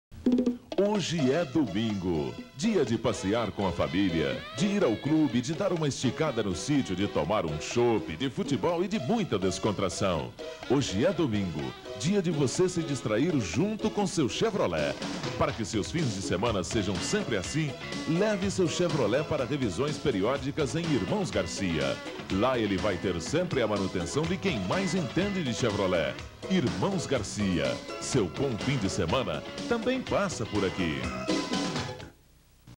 Programa Close - Comercial Irmãos Garcia
Uma das primeiras concessionárias de automóveis de Uberlândia, inaugurada nos anos de 1930, foi a ABC Irmãos Garcia. Era representante da marca Chevrolet.

Nesse comercial, gravado na década de 1990, o objetivo era divulgar os serviços de manutenção que a empresa oferecia ao mercado na época.